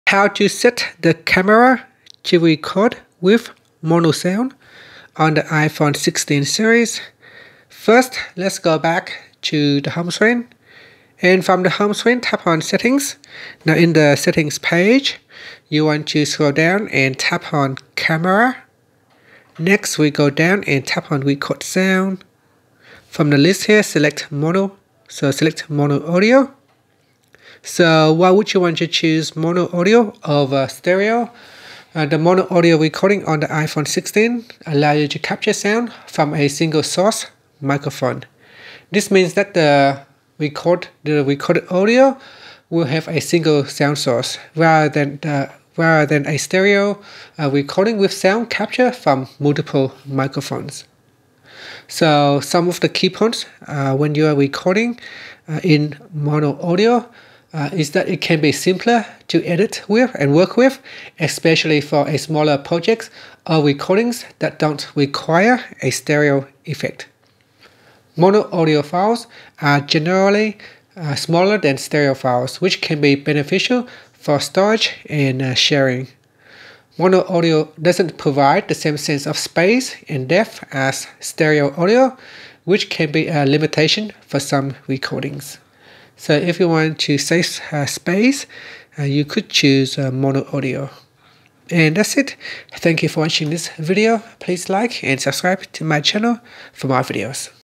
🔊 How to Set Mono Sound for Video Recording on iPhone 16 Series | iOS 18 Tutorial
In this ITJungles, learn how to set the camera to record video with mono sound on your iPhone 16, iPhone 16 Pro, iPhone 16 Pro Max, or iPhone 16 Plus, running iOS 18. I will walk you through the simple steps to adjust your iPhone's audio settings for a mono-recording experience, this is perfect for users who prefer unified sound channels.

Mono sound is a type of audio recording that captures sound from a single source. This is different from stereo sound, which captures sound from two separate sources, creating a more immersive experience. On an iPhone, using mono sound can be beneficial for simpler setups, reducing background noise, creating smaller file sizes, and focusing on the main subject of your video. I personally would always choose to use stereo sound, but if you prefer you can select the mono option.